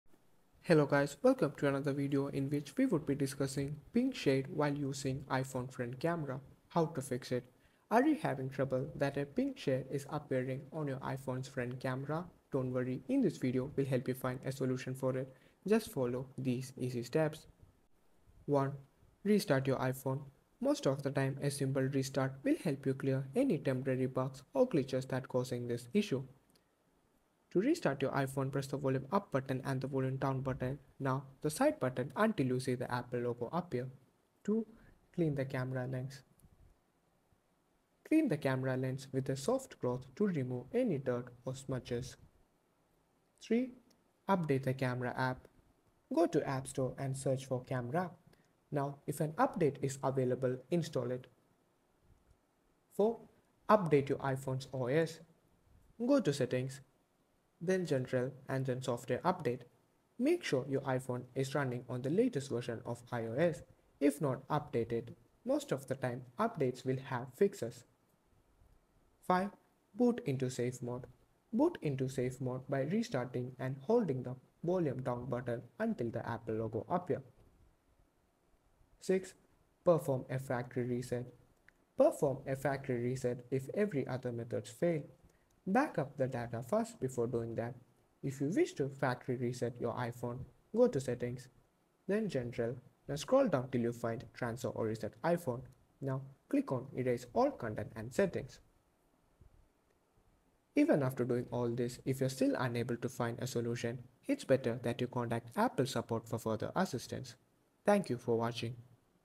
Pink Shade While Using iPhone Front Camera: Here’s How To Fix
Seeing a pink tint on your iPhone’s front camera? Here’s how to troubleshoot and fix the issue.

0:00 - Introduction
0:20 - Restart Your Device
0:35 - Clean Camera Lens
0:46 - Update Camera App
0:55 - Update iOS
1:12 - Boot Into Safe mode
1:21 - Perform  A Factory Reset
1:41 - Contact Apple Support